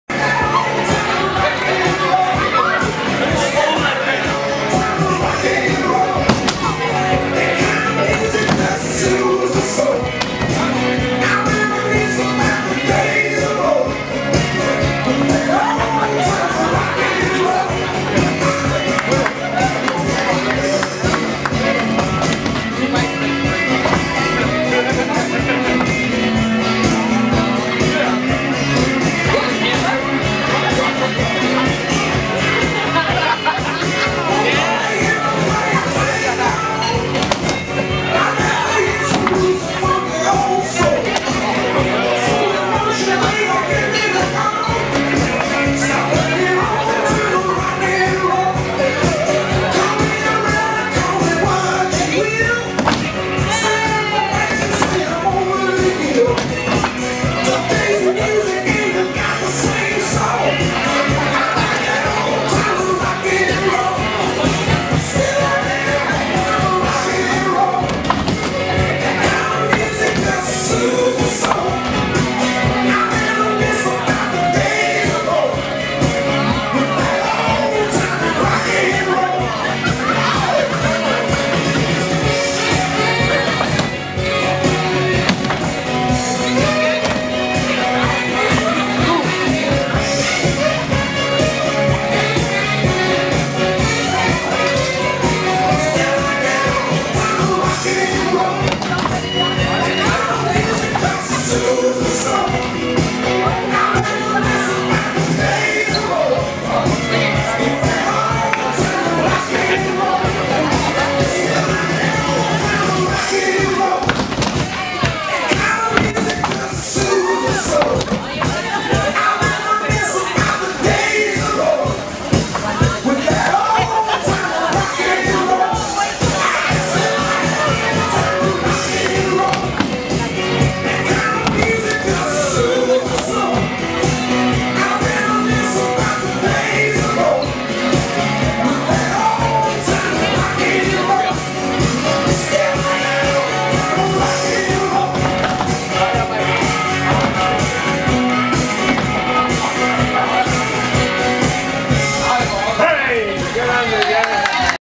Wee Waa Debutante Ball fun dance
Wee Waa Debutante Ball fun dance Saturday August 26 at Wee Waa Bowling Club - eight debutantes and their partners along with instructors Lyle and Helen Cathcart